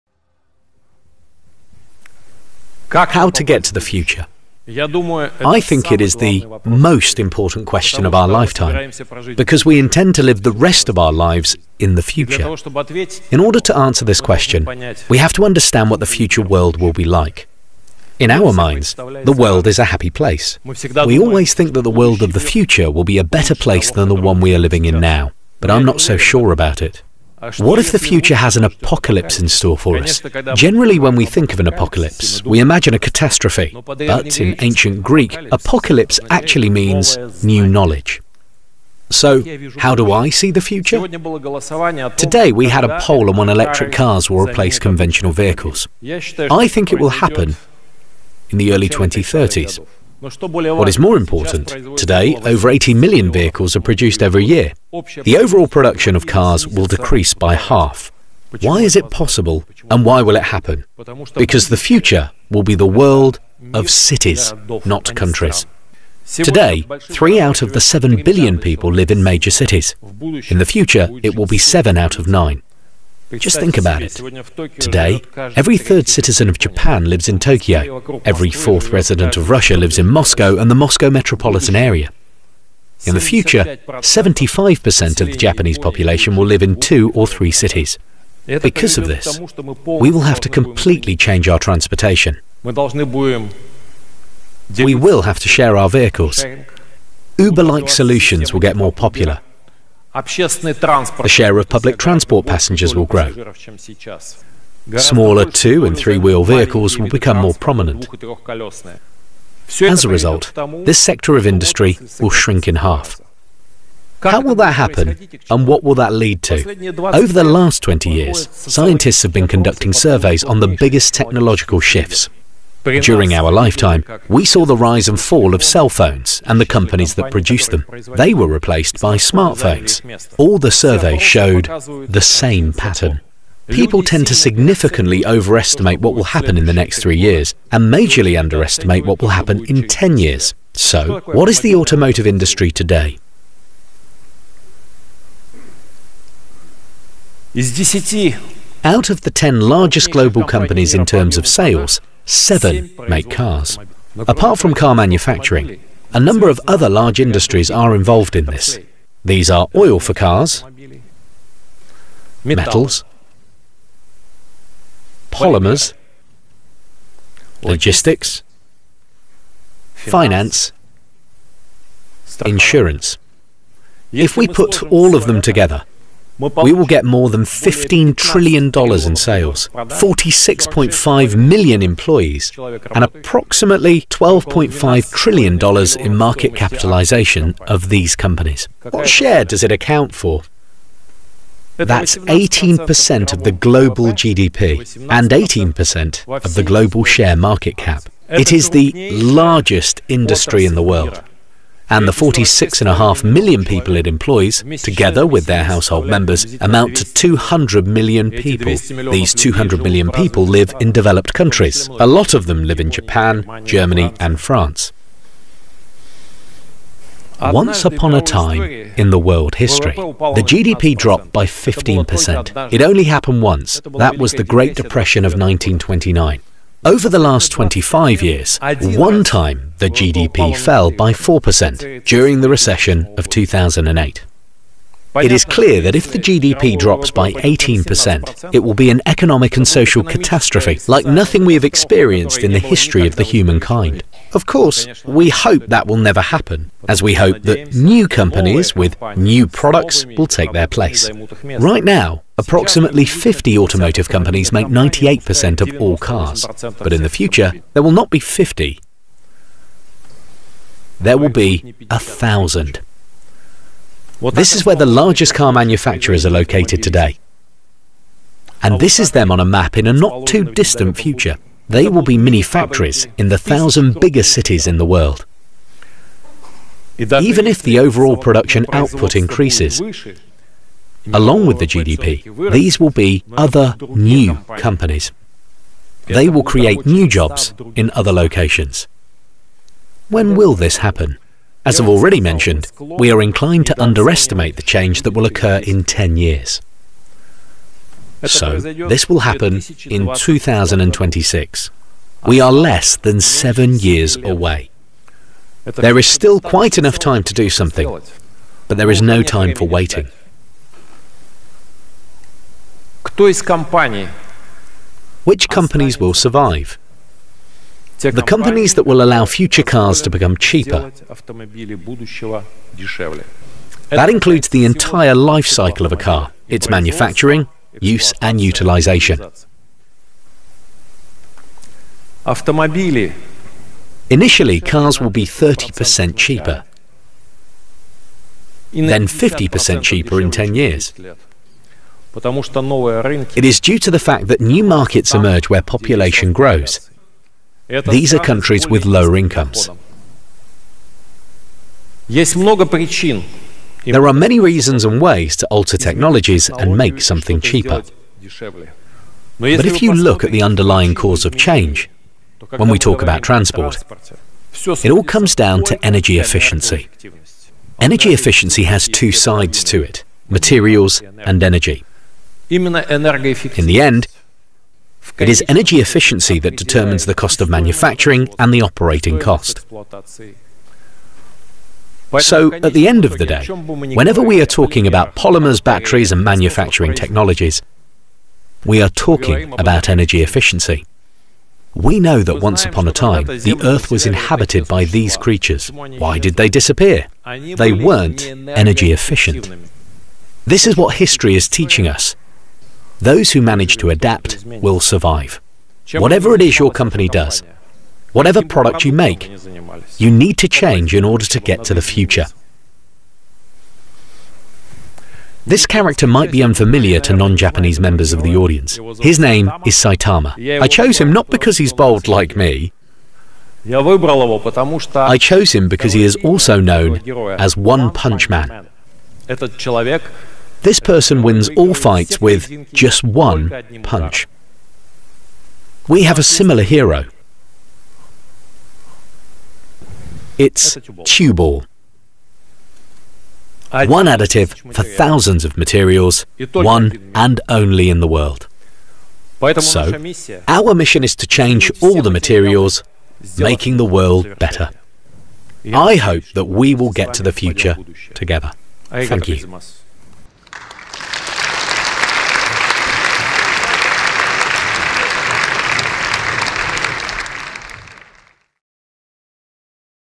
How to get to the Future (Yury Koropachinskiy, OCSiAl)
What will the world of the future be like? Where will we live? What forms of transport will we use? Yury Koropachinskiy, President and Co-founder of the world's largest graphene nanotube manufacturer, addresses the future of mobility, which will depend to a great extent on advances in materials technology.

The Nanoaugmented Materials Industry Summit, Kyoto, November 2019